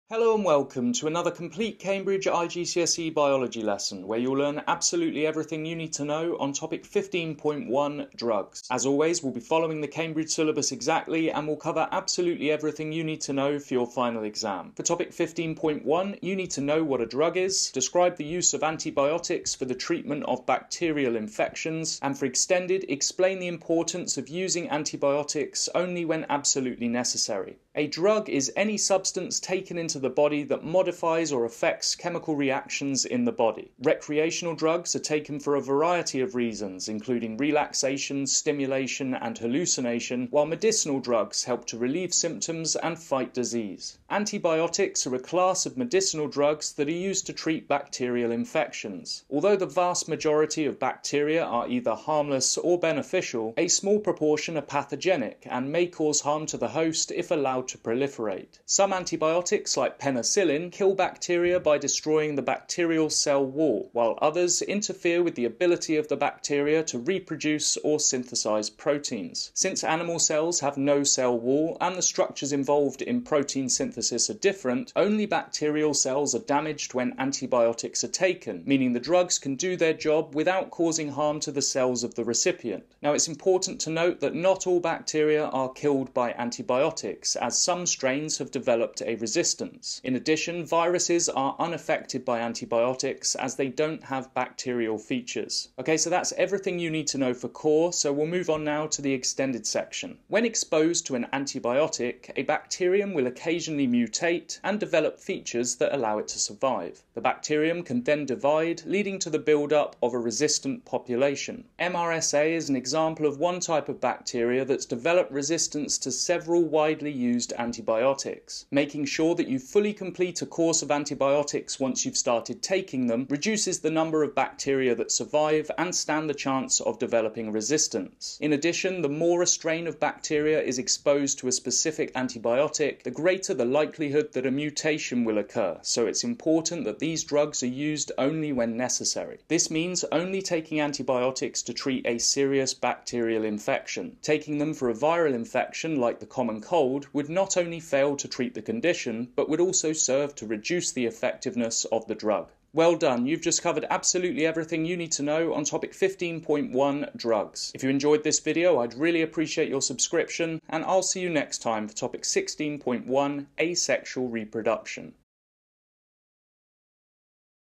IGCSE Biology - Drugs (15.1)
IGCSE Chemistry - The Complete Course:
🎓 Complete Teaching Resource (including past exam questions and mark schemes organised by chapter)

Cambridge IGCSE Biology (0610/0970)
Chapter 15 - Drugs
Topic 15.1 - Drugs
For exams from 2025 (core and extended)

As always this video follows the Cambridge syllabus exactly and contains absolutely everything you need to know for your final exam!

My other channels:
@igpecomplete  - Cambridge IGCSE PE
@ocrpecomplete  - OCR GCSE PE

Timestamps:
0:00 Contents
0:27 Drugs
0:44 Antibiotics
1:38 Antibiotic resistance (extended)

Core content:
- Describe a drug as any substance taken into the body that modifies or affects chemical reactions in the body
- Describe the use of antibiotics for the treatment of bacterial infections
- State that some bacteria are resistant to antibiotics which reduces the effectiveness of antibiotics
- State that antibiotics kill bacteria but do not affect viruses

Extended content:
- Explain how using antibiotics only when essential can limit the development of resistant bacteria such as MRSA